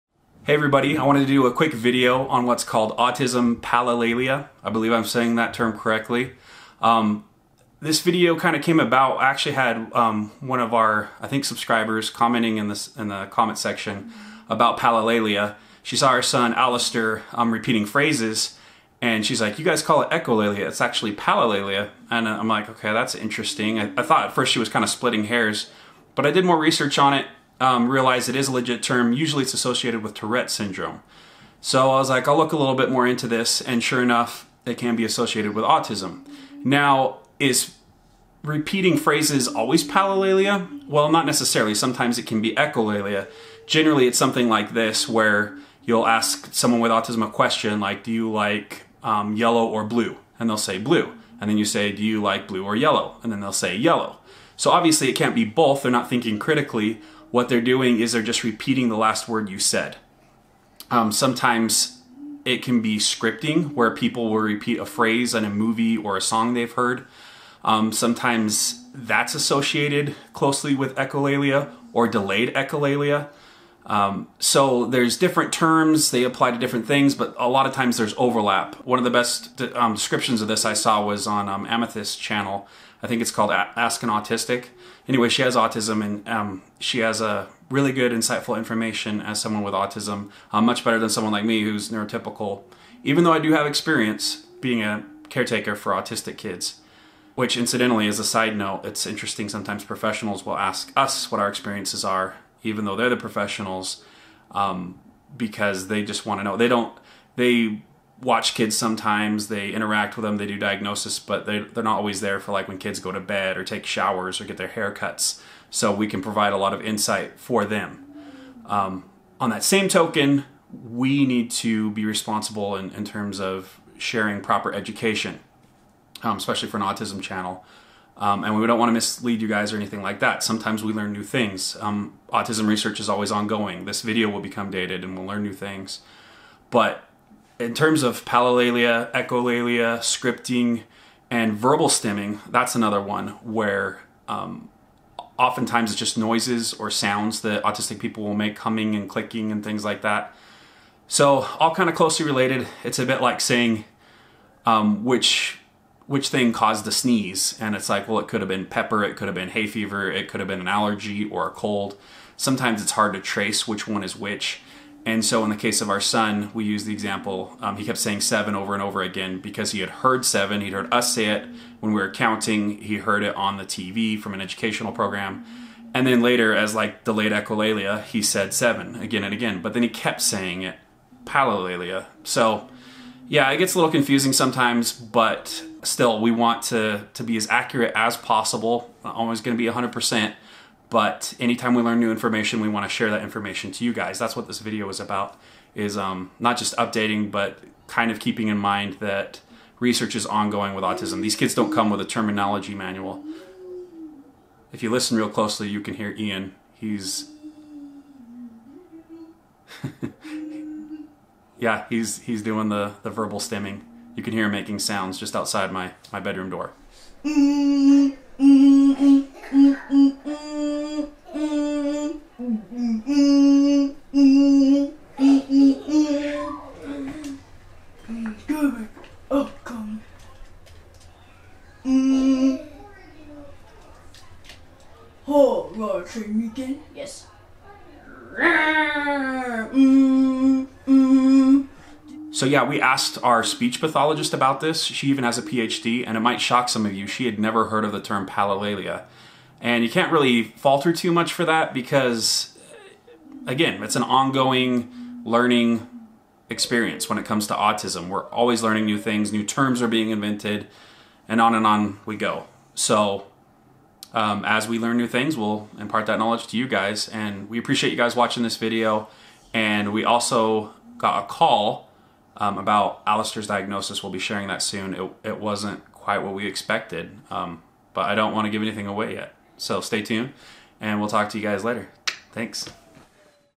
Autism Palilalia?
There are many terms for autistic behaviors. In this video we cover Echolalia, Palilalia, Scripting and Vocal Stimming.